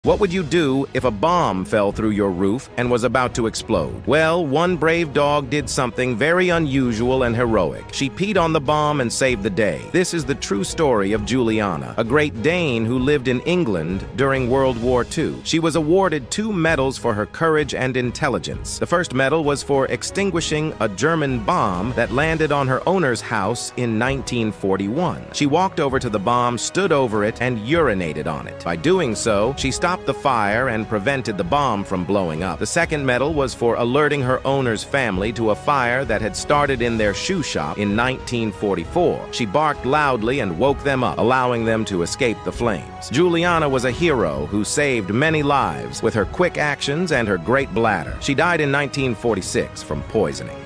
How Juliana the Great Dane Saves the Day with Her Pee
Meet Juliana, the brave Great Dane who saved lives with her heroic actions during World War II! From peeing on a bomb to alerting her family of a fire, this extraordinary dog was awarded two medals for her courage and intelligence.